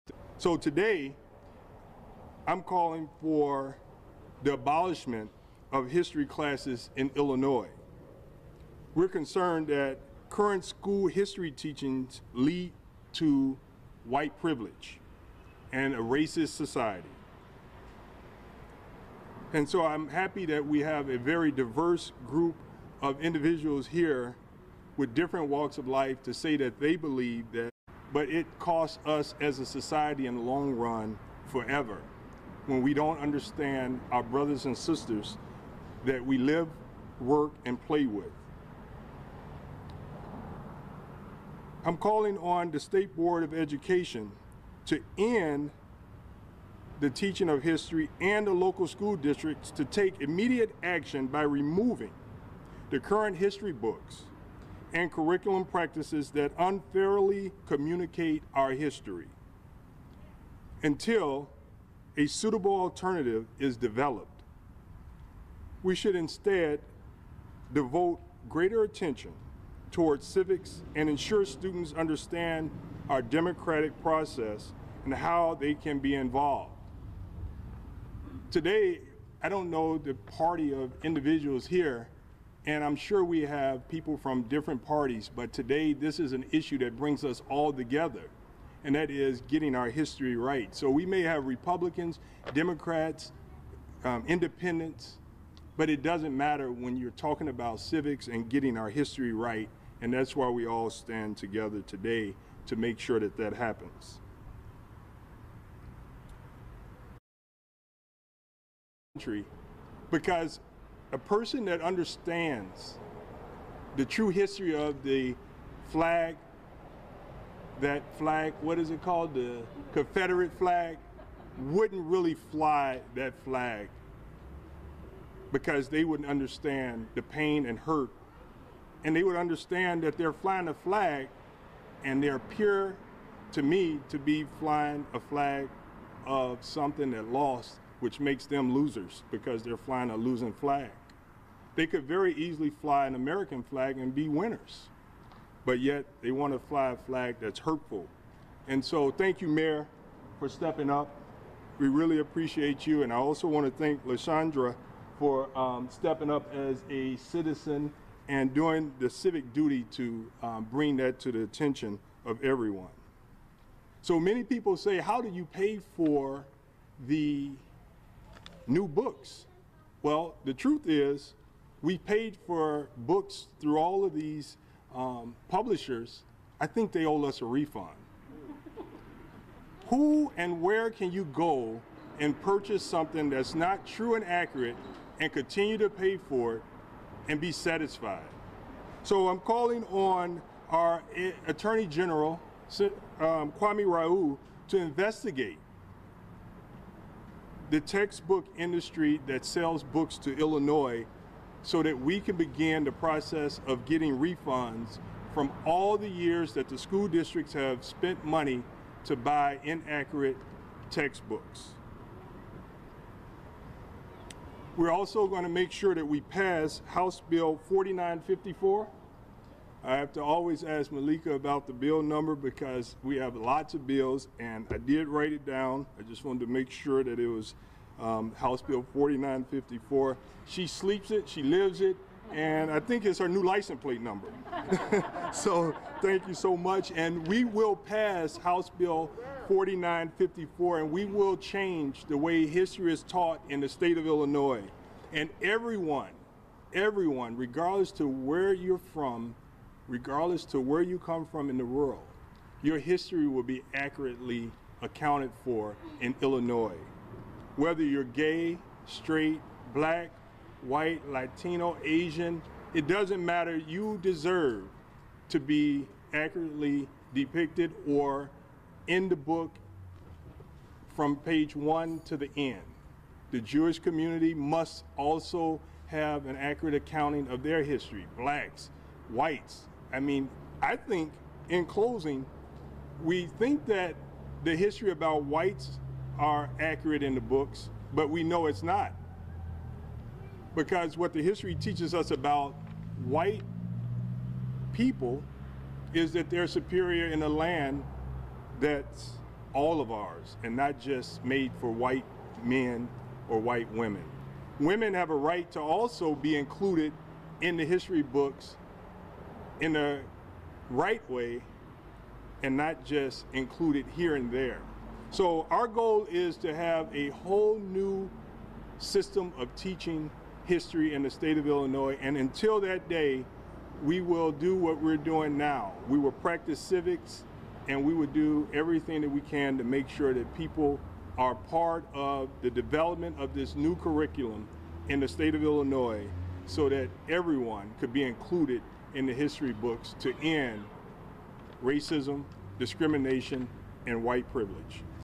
La Shawn Ford (D-Chicago) wants to abolish history classes in Illinois
State rep. La Shawn Ford (D) Illinois, 8th district is trying to abolish history classes in schools stating “We are concerned that current history school teaching lead to white privilege and a racist society”

State Representative La Shawn Ford (D-Chicago) said Sunday he wants to abolish history classes in Illinois until a new curriculum is developed.